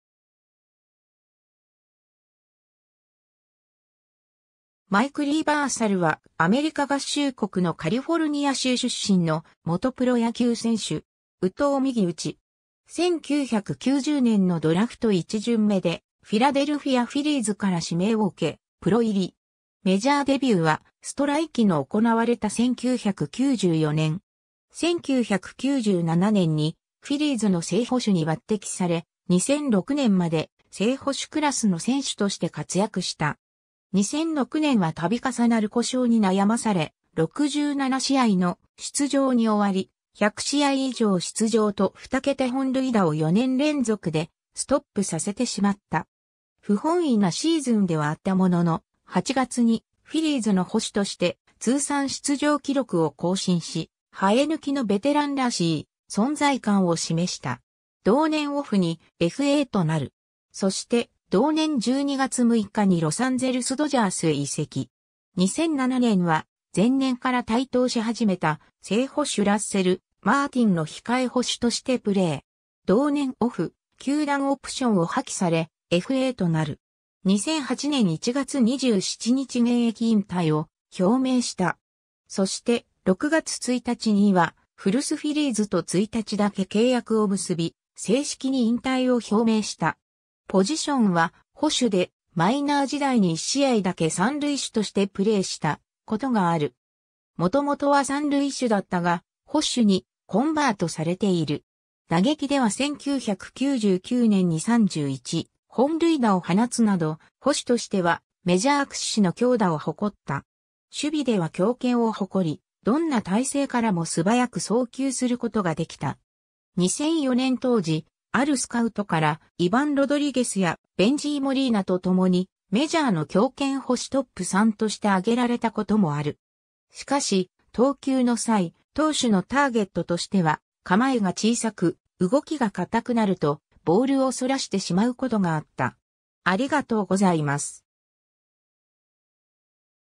マイク・リーバーサル
マイク・リーバーサル（Michael Scott Lieberthal, 1972年1月18日 - ）は、アメリカ合衆国のカリフォルニア州出身の元プロ野球選手（捕手）。
右投右打。
 1990年のドラフト1巡目でフィラデルフィア・フィリーズから指名を受け、プロ入り。
メジャーデビューは、ストライキの行われた1994年。
1997年に、フィリーズの正捕手に抜擢され、2006年まで正捕手クラスの選手として活躍した。
 2006年は度重なる故障に悩まされ、67試合の出場に終わり、100試合以上出場と2桁本塁打を4年連続でストップさせてしまった。
不本意なシーズンではあったものの、8月にフィリーズの捕手として通算出場記録（2006年終了時点で1139試合）を更新し、生え抜きのベテランらしい存在感を示した。
同年オフにFAとなる。
そして、同年12月6日にロサンゼルス・ドジャースへ移籍。
 2007年は、前年から台頭しはじめた正捕手ラッセル・マーティンの控え捕手としてプレー。
同年オフ、球団オプションを破棄され、FAとなる。
2008年1月27日現役引退を表明した。
 そして、6月1日には古巣フィリーズと1日だけ契約を結び、正式に引退を表明した。
 ポジションは捕手で、マイナー時代に1試合だけ三塁手としてプレーしたことがある。
元々は三塁手だったが、捕手にコンバートされている。
 打撃では1999年に31本塁打を放つなど、捕手としてはメジャー屈指の強打を誇った。
 守備では強肩を誇り、どんな体勢からも素早く送球することができた。
2004年当時、あるスカウトから、イバン・ロドリゲスやベンジー・モリーナと共に、メジャーの強肩捕手トップ3として挙げられたこともある。
しかし、投球の際、投手のターゲットとしては構えが小さく、動きが硬くなるとボールを逸らしてしまうことがあった。